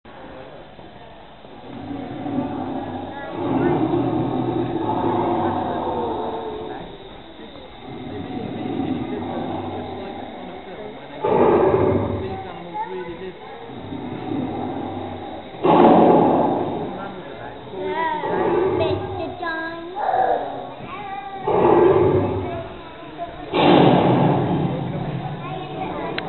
Tyrannosaur
Tyrannosaurus toy at Natural History Museum London